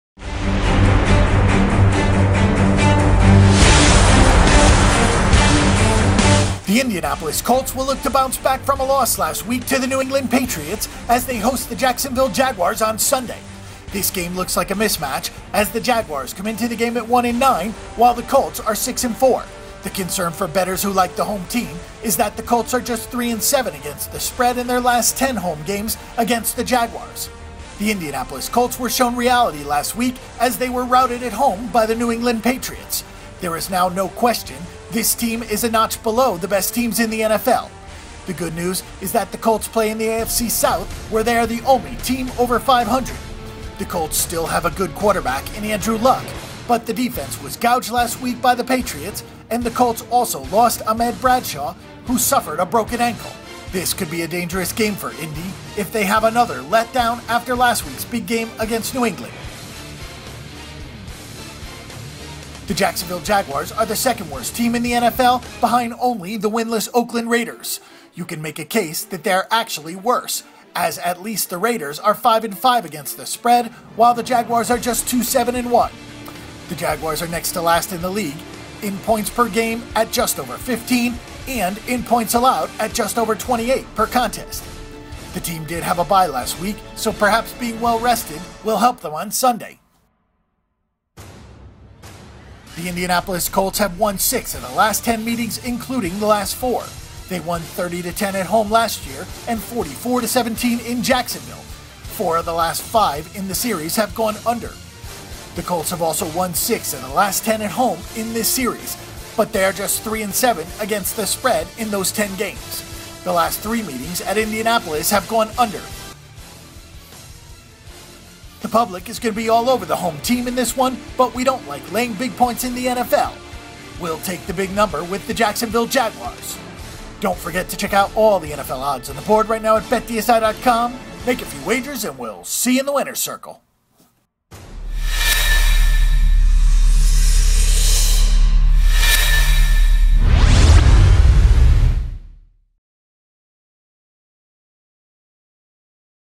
Jacksonville Jaguars vs Indianapolis Colts Odds | NFL Picks
Game Notes

The Indianapolis Colts have won six of the last ten meetings including the last four. They won 30-10 at home last year and 44-17 in Jacksonville. Four of the last five in the series have gone under. The Colts have also won six of the last ten at home in this series but they are just 3-7 ATS in those games. The last three meetings at Indianapolis have gone under.

NFL Predictions

The public is going to be all over the home team in this one but I don’t like laying big points in the NFL. I will take the big number with the Jacksonville Jaguars.

Check out the latest NFL lines at the online sportsbook.